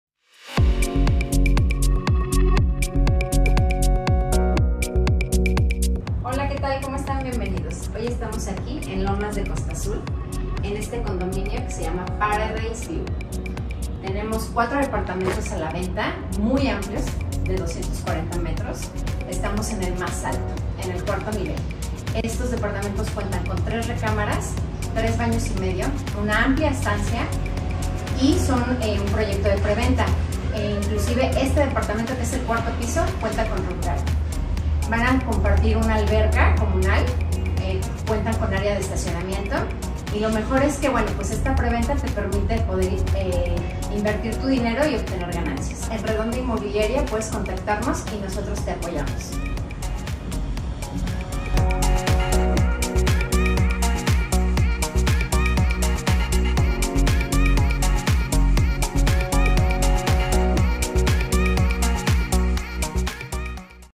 Excelente condominio con 4 departamentos en preventa: Paradise View, Lomas de Costa Azul, Acapulco
📍 Paradise View, Lomas de Costa Azul, Acapulco

▶️ Condominio con 4 departamentos en preventa

🛏️ 3 recamaras
🛀 3.5 baños
🚗 Estacionamiento

Desde 💲4,250,000.00